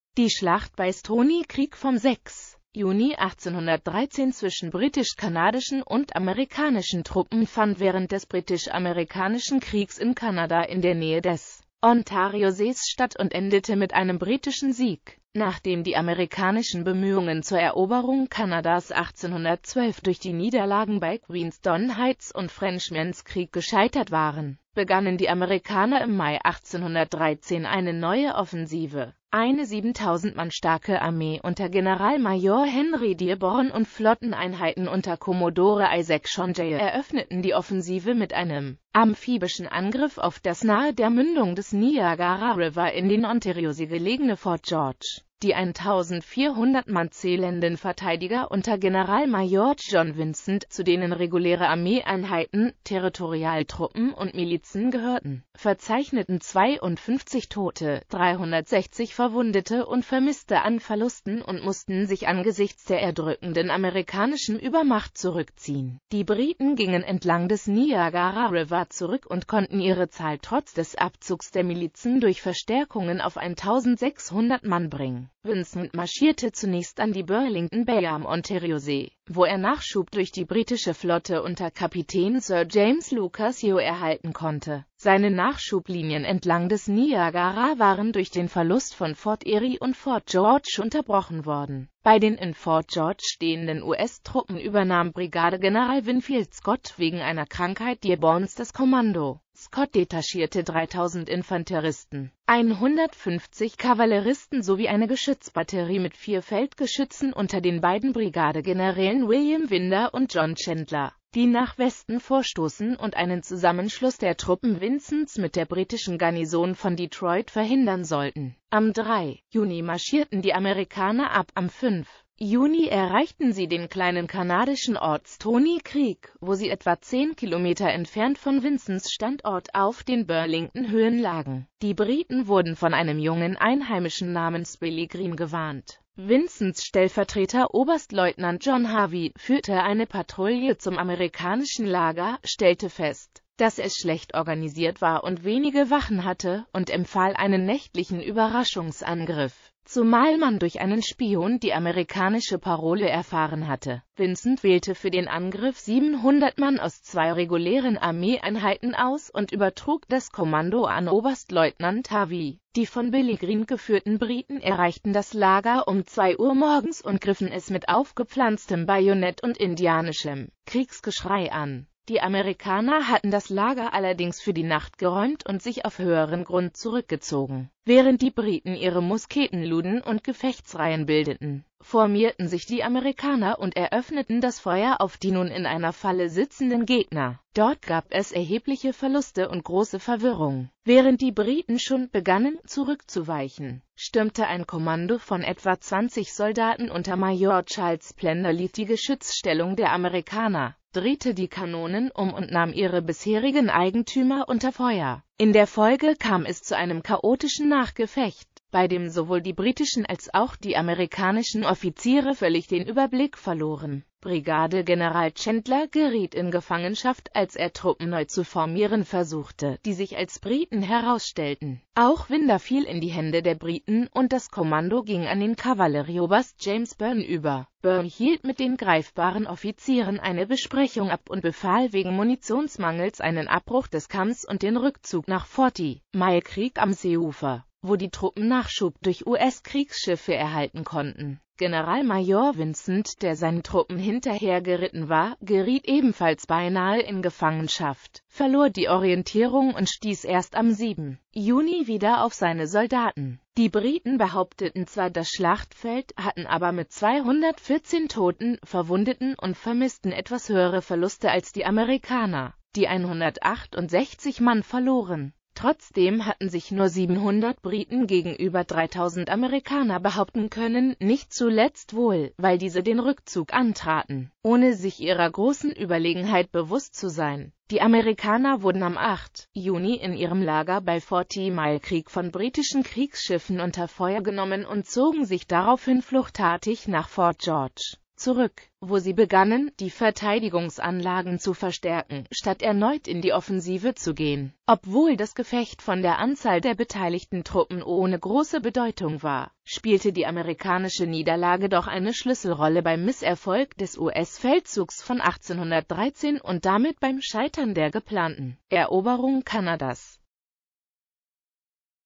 Schlacht bei Stoney Creek
Die Schlacht bei Stoney Creek vom 6.Juni 1813 zwischen britisch-kanadischen und amerikanischen Truppen fand während des Britisch-Amerikanischen Kriegs in Kanada in der Nähe des Ontariosees statt und endete mit einem britischen Sieg.Nachdem die amerikanischen Bemühungen zur Eroberung Kanadas 1812 durch die Niederlagen bei Queenston Heights und Frenchman’s Creek gescheitert waren, begannen die Amerikaner im Mai 1813 eine neue Offensive.Eine 7.000 Mann starke Armee unter Generalmajor Henry Dearborn und Flotteneinheiten unter Kommodore Isaac Chauncey eröffneten die Offensive mit einem amphibischen Angriff auf das nahe der Mündung des Niagara River in den Ontariosee gelegene Fort George.